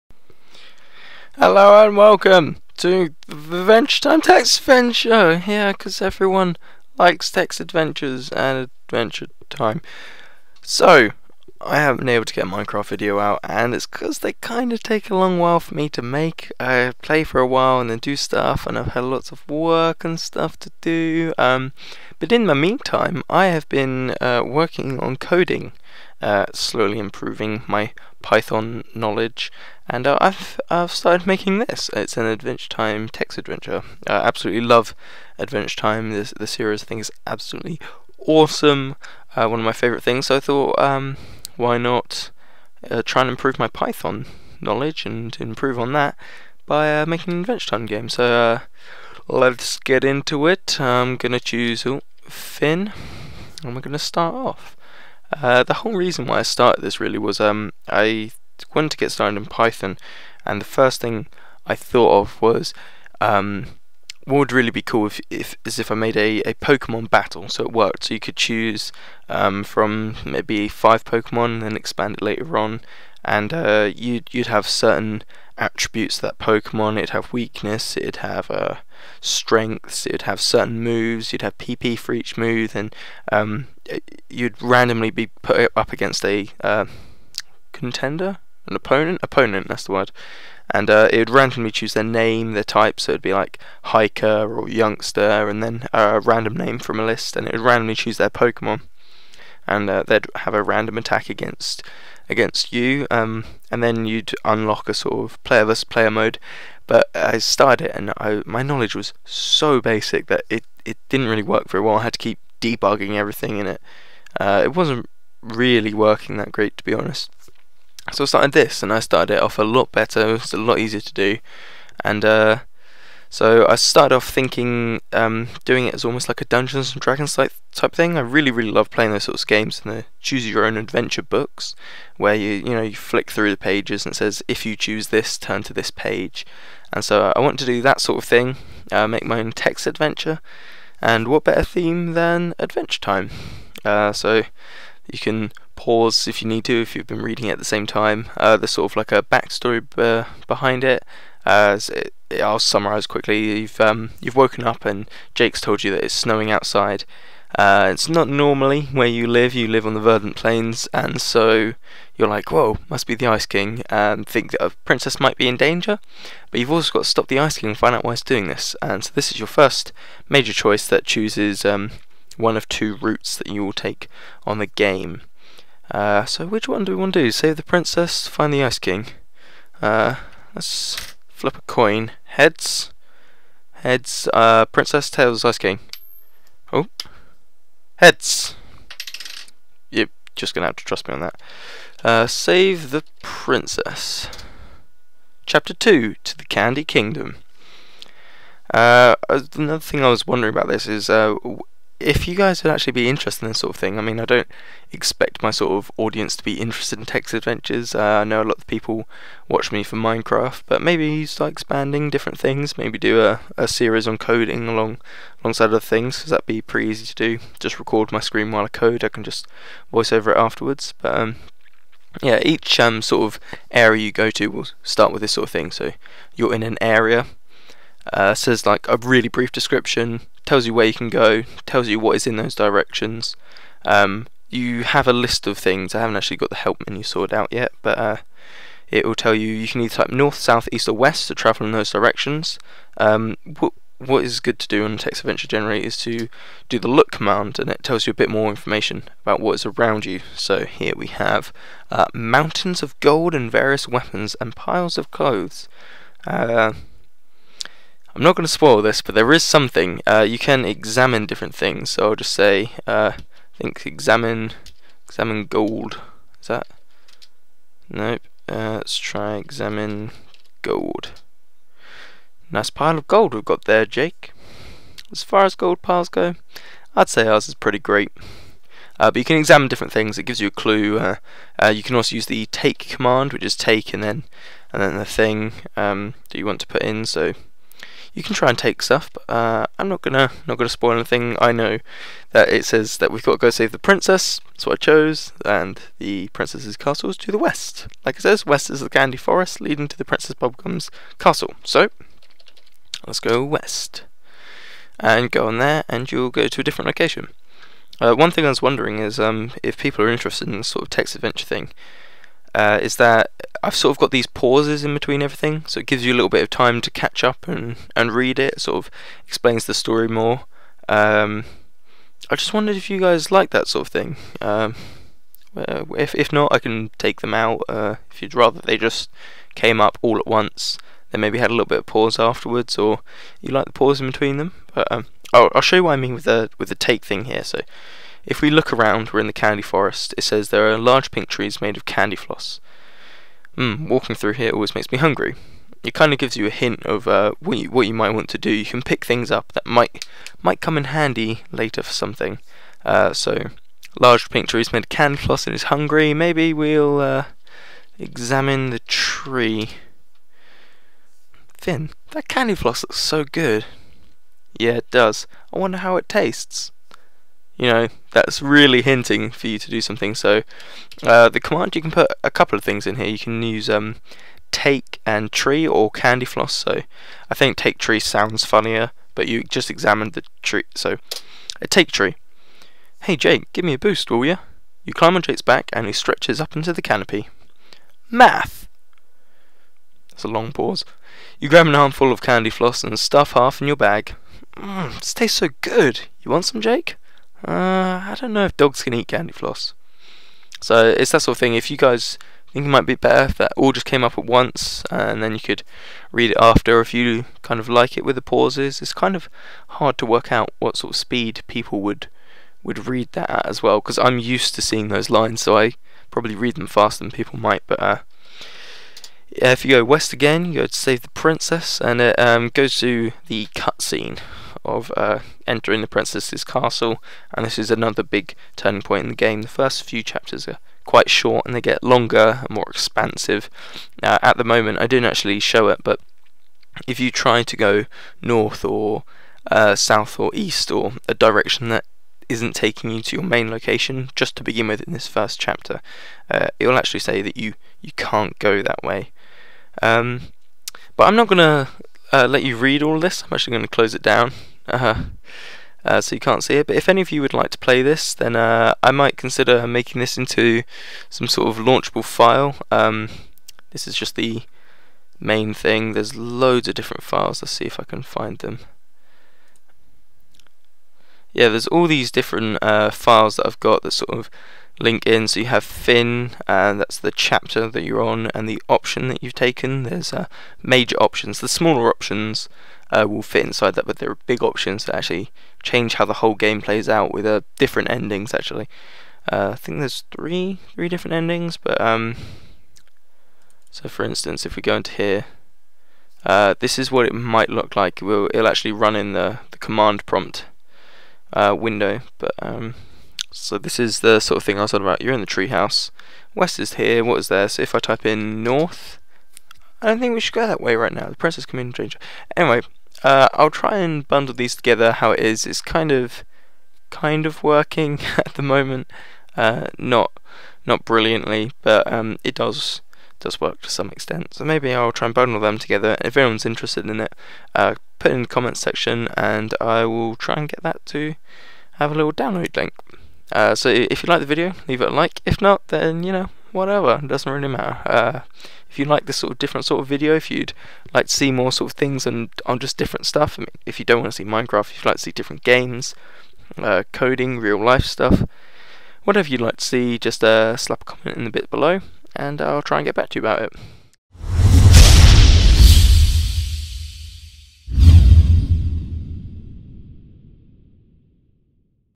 Adventure Time Text Adventure Showcase
Not really able to get any Minecraft videos out right now, I have half an episode recorded, but I don't feel like I can put it out yet and I also need to edit it all - which takes a lot of time.
I was working on this and thought I could make a quick 'filler' video about coding and this game thing that I am making
If anyone would like to see more of this sort of coding thing, or play the game as it is now (which isn't very good at all haha) then comment below or leave me a message!